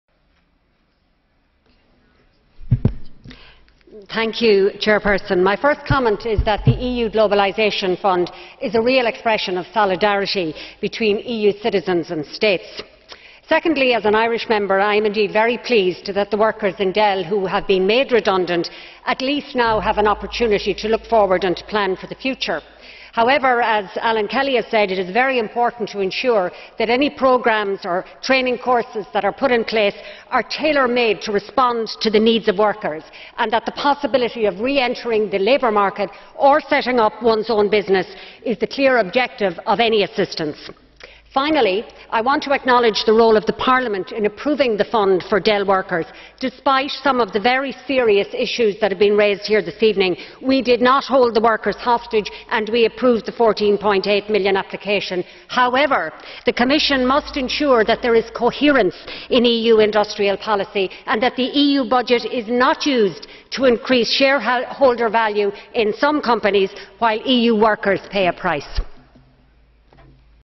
Marian Harkin on Mobilisation of the European Globalisation
Speech by Marian Harkin MEP (ALDE-ADLE) on :
Mobilisation of the European Globalisation Adjustment Fund : Belgium - textile industry; Ireland - Dell - Relocation of undertakings in the EU and the role of EU financial instruments

Harkin_091124_212747_mesp_en